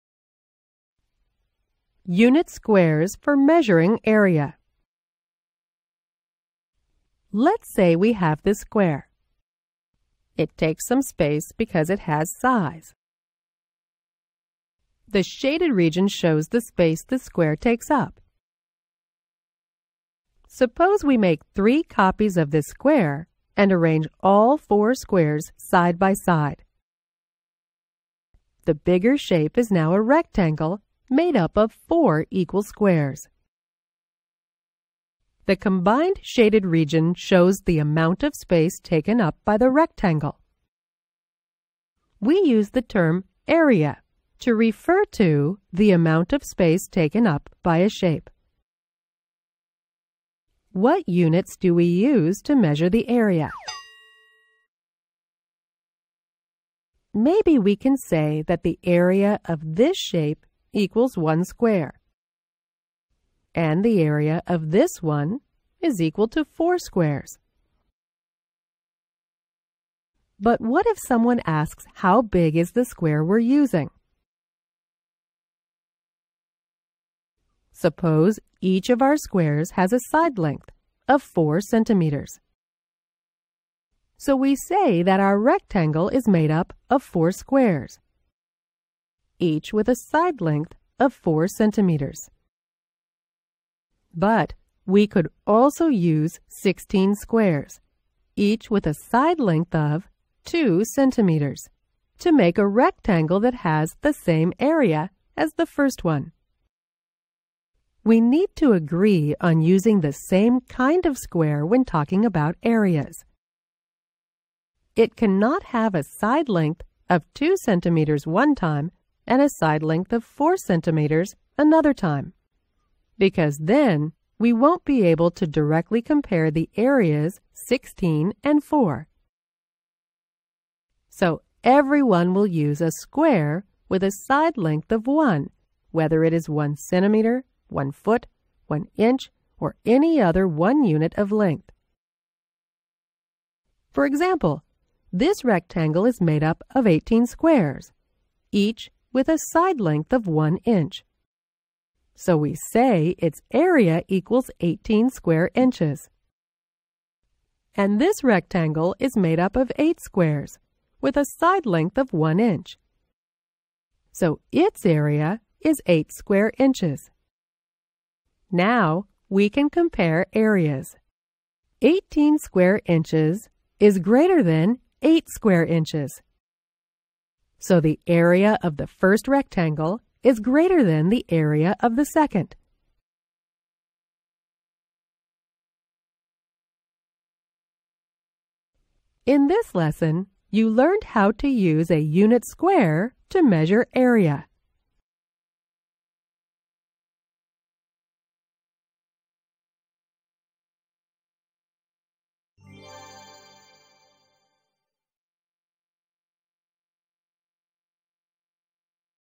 Unit Square for Measuring Area
Understand that an area of a figure is the number of unit squares that make up that figure.

Now free and open for everyone.